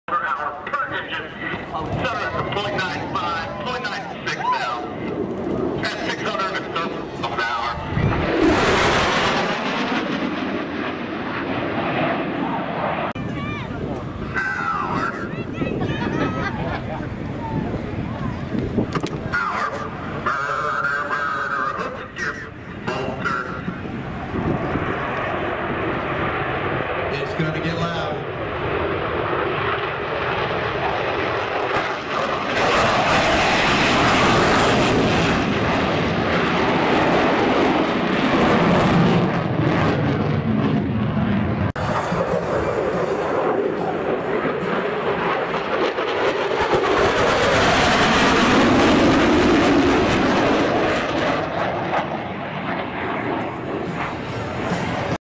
F/A-18 Rhino Demo Team Highlights Central Coast Airfest Santa Maria CA Sep-22-2024
US Navy F/A-18 Super Hornet Demo Team performance included high speed pass, touch and go takeoff and dedication pass.